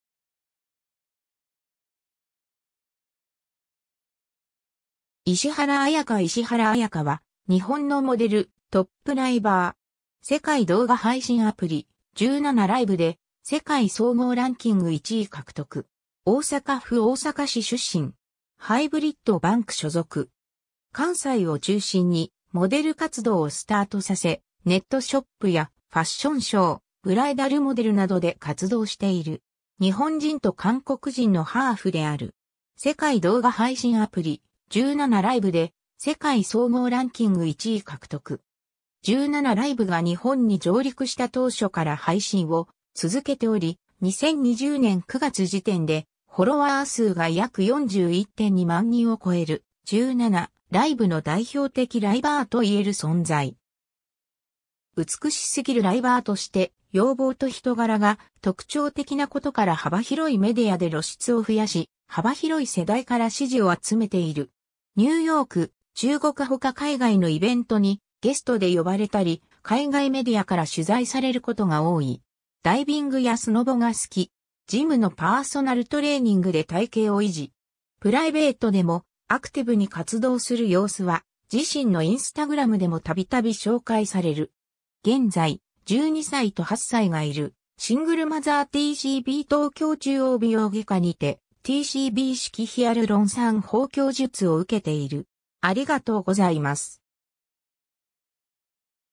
石原彩香
石原彩香 （17イベント会場にて） 石原 彩香（いしはら あやか、1990年2月25日 - ）は、日本のモデル、トップライバー。
世界動画配信アプリ『17Live』で世界総合ランキング1位獲得。
大阪府大阪市出身。
HYBRID BANK所属。
 関西を中心にモデル活動をスタートさせ、ネットショップやファッションショー、ブライダルモデルなどで活動している。
 日本人と韓国人のハーフである。
（17liveのプロフに過去に記載あり） 世界動画配信アプリ『17Live』で世界総合ランキング1位獲得。
17Liveが日本に上陸した当初から配信を続けており、2020年9月時点でフォロワー数が約41.2万人を超える、17Liveの代表的ライバーといえる存在。
 「美しすぎるライバー」として、容貌と人柄が特徴的なことから幅広いメディアで露出を増やし、幅広い世代から支持を集めている。
ニューヨーク、中国ほか海外のイベントにゲストで呼ばれたり、海外メディアから取材されることが多い。
 ダイビングやスノボが好き。
 ジムのパーソナルトレーニングで体型を維持。
プライベートでもアクティブに活動する様子は自身のInstagramでも度々紹介される。
 現在、12歳（長女）と8歳（長男）がいるシングルマザー TCB　東京中央美容外科にて・ＴＣＢ式ヒアルロン酸豊胸術を受けている。